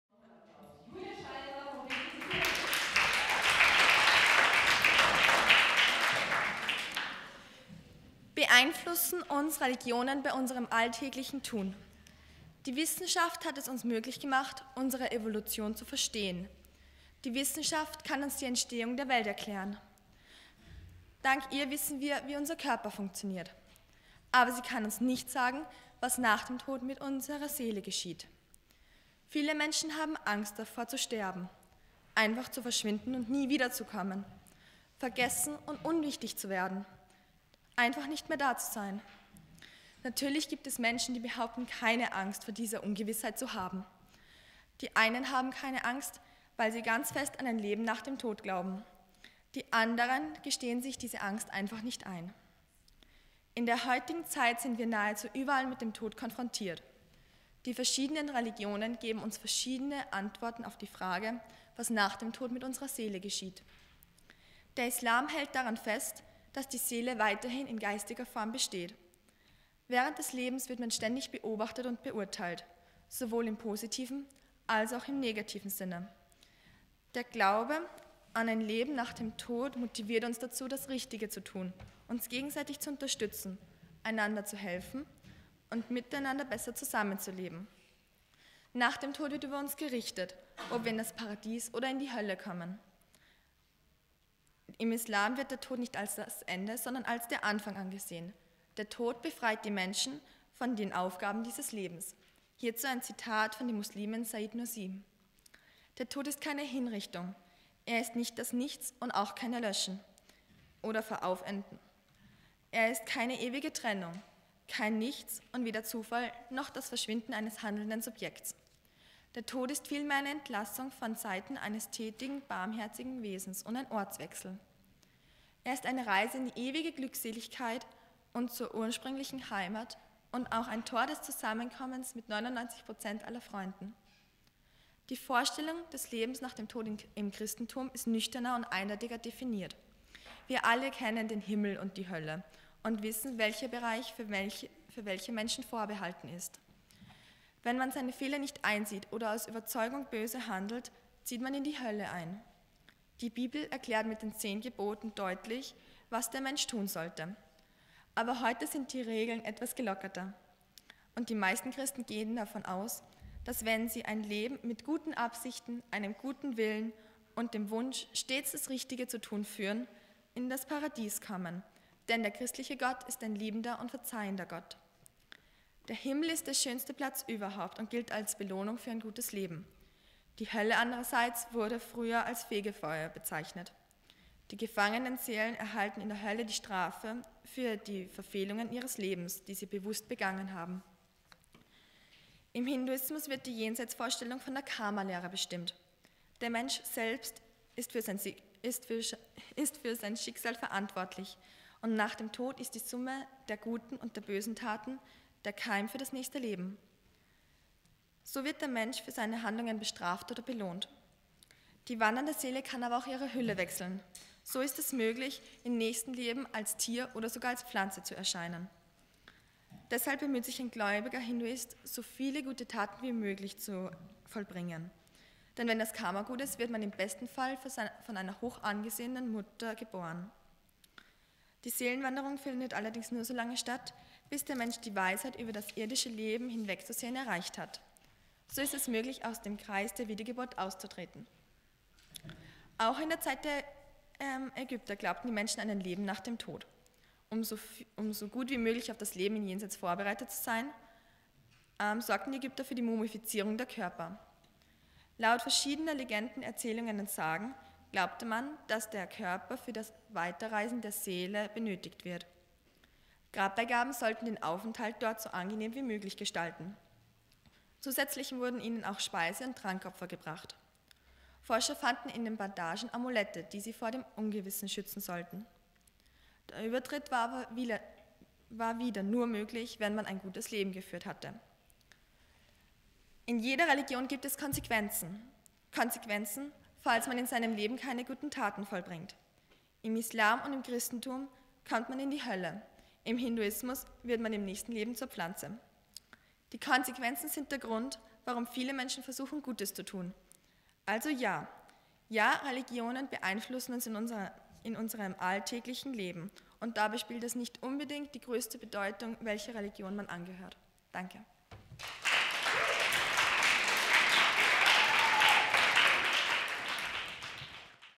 Julia Scheidler | Jugendredewettbewerb 2016 | FS1
Julia Scheidler vom Jugendredewettbewerb 2016 mit "Das Leben nach dem Tod".